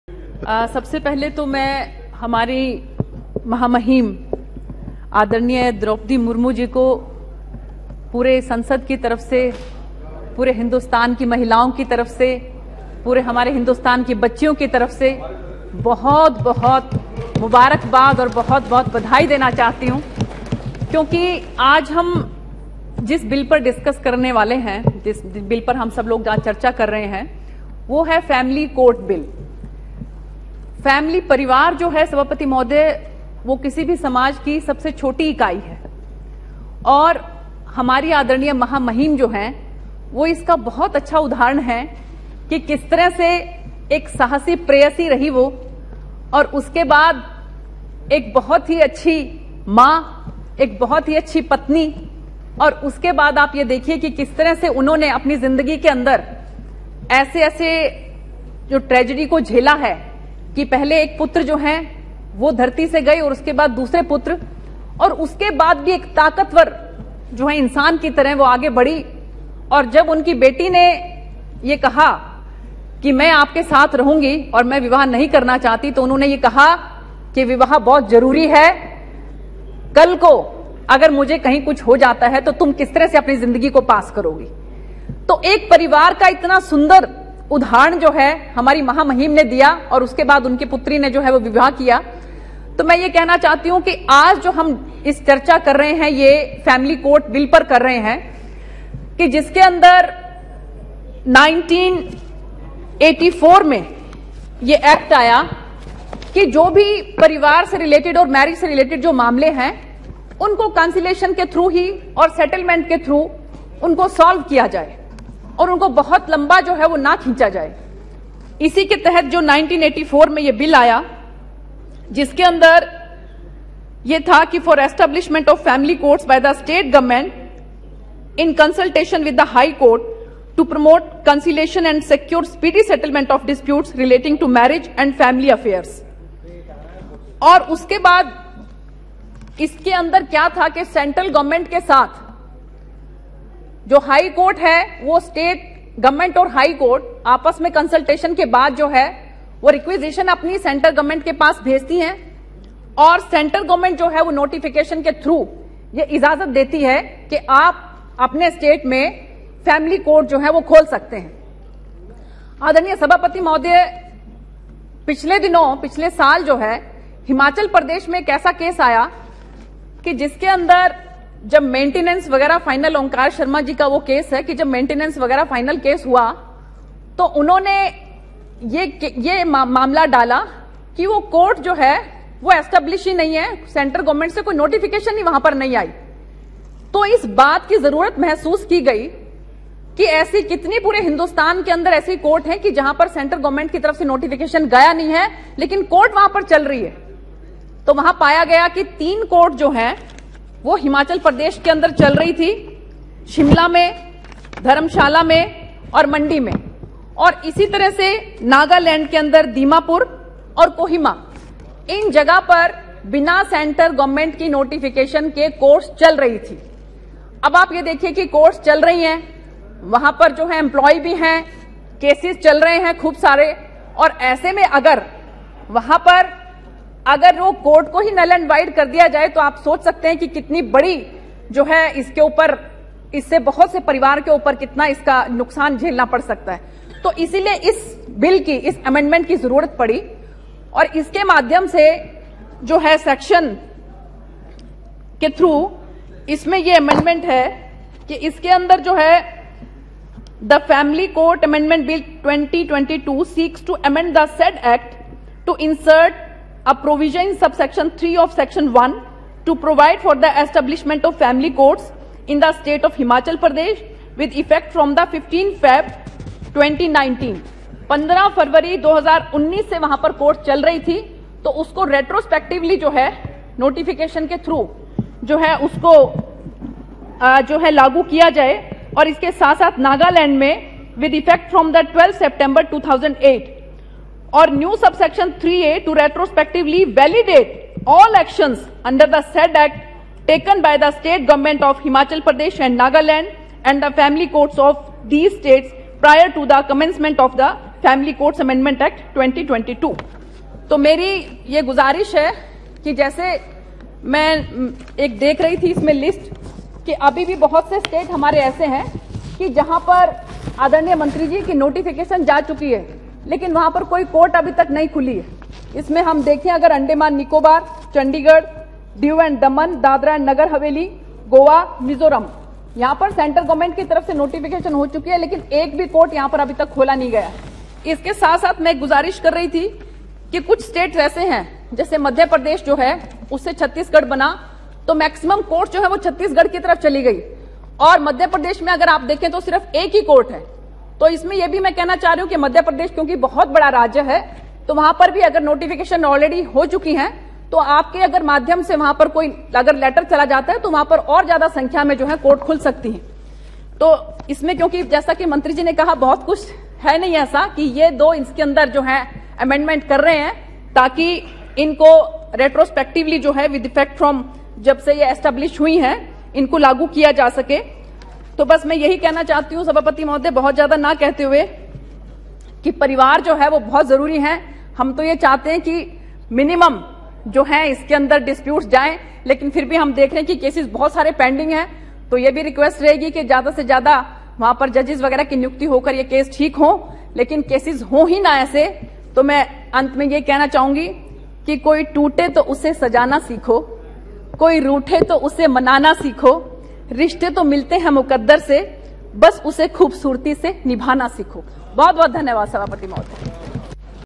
Sunita Duggal ने आज सदन में Droupadi Murmu को लेकर बोली ऐसी बात जमकर बजी तालियां Congress बोलती बंद!
Sunita Duggal’s Remarks Family Courts Amendment Bill, 2022, 25 July, 2022